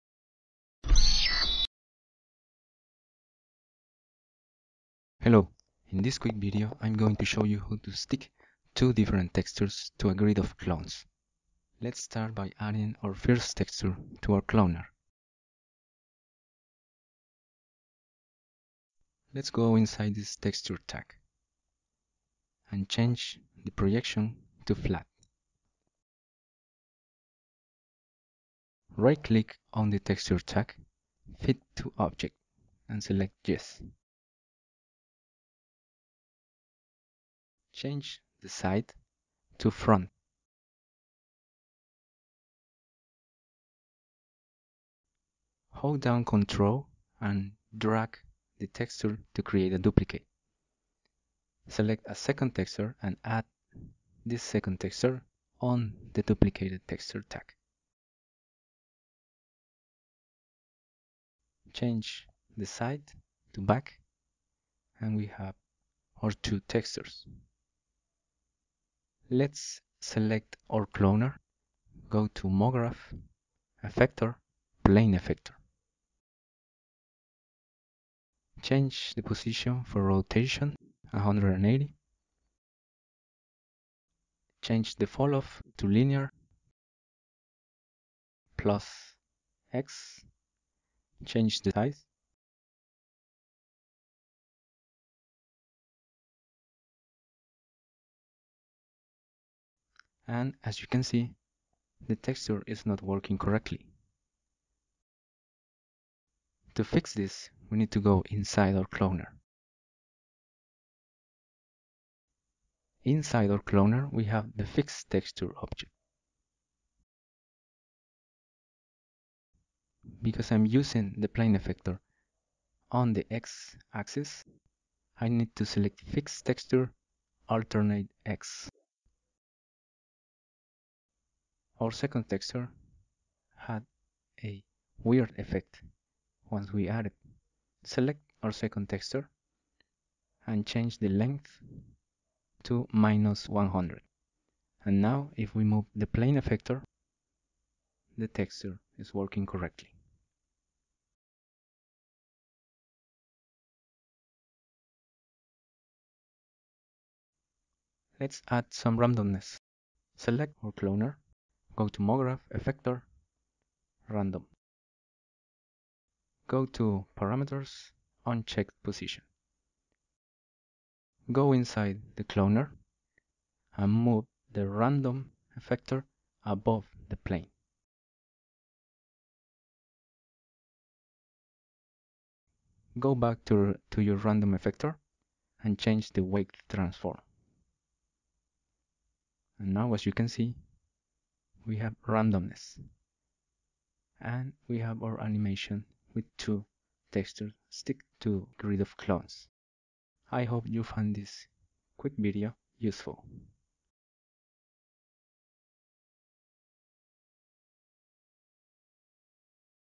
C4D Tip: Sticking Two Textures to Clones
In this quick video i'm going to show you how to stick two textures to a grid of clones, and make the textures react correctly when you use Effectors.